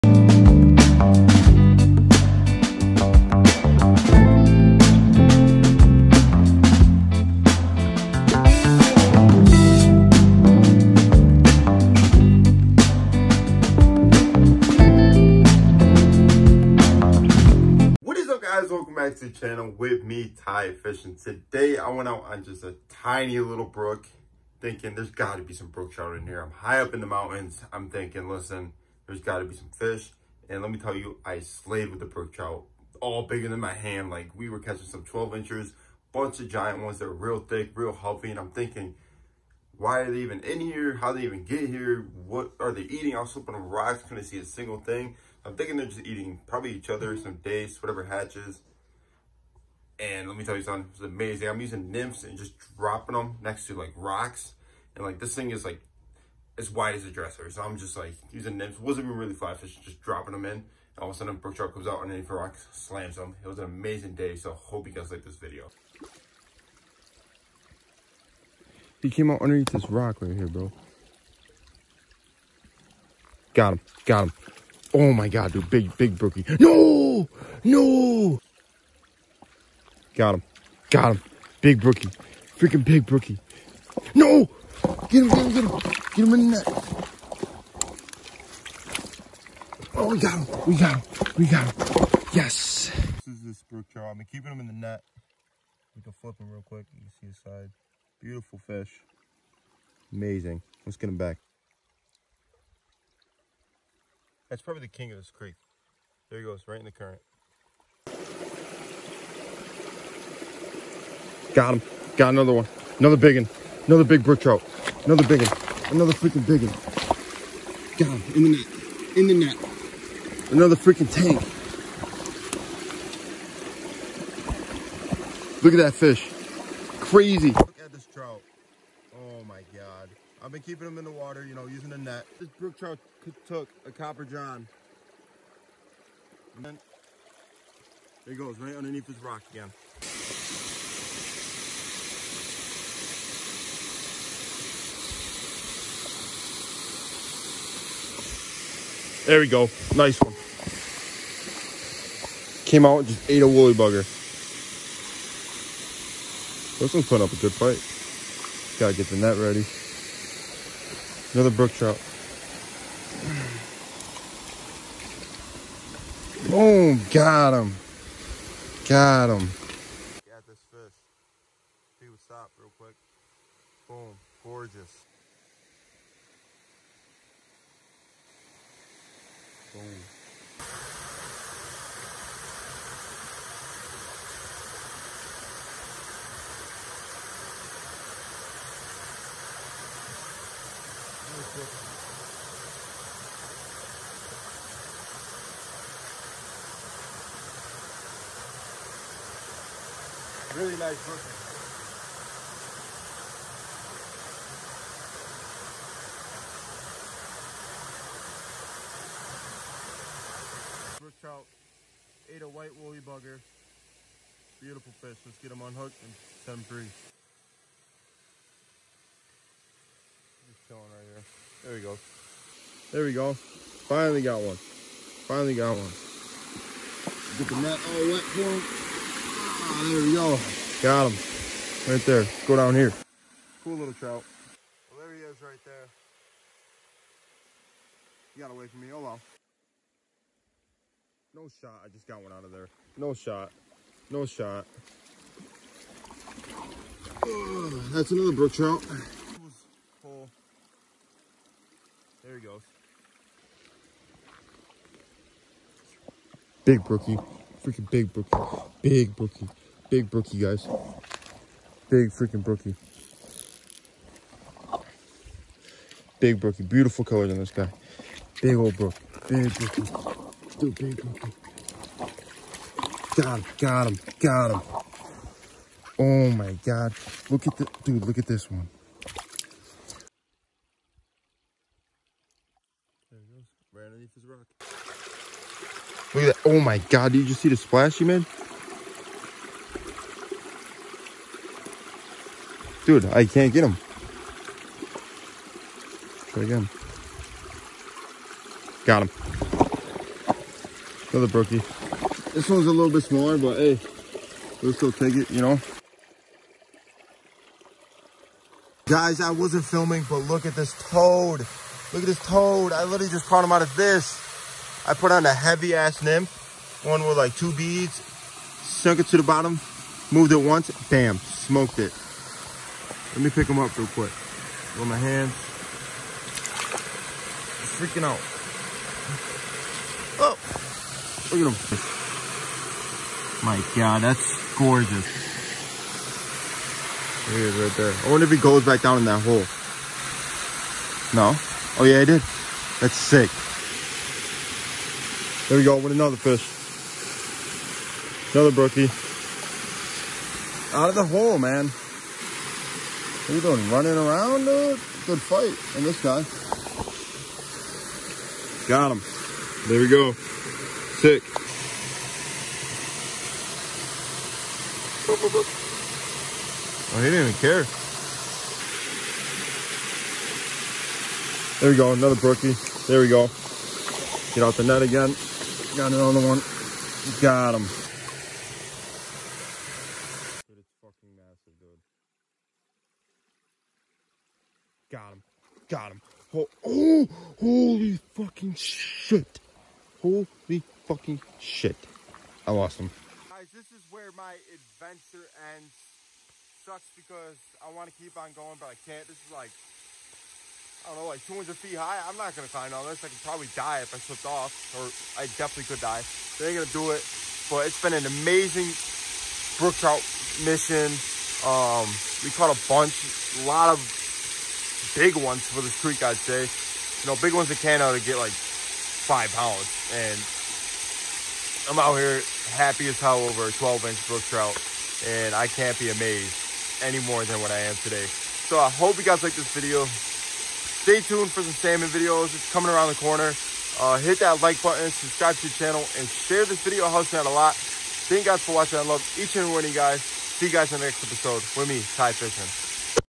Tiny stream holds BIG BROOK TROUT!!!
Went out on a on a little journey today looking for some native brook trout and i found the holy grail!!!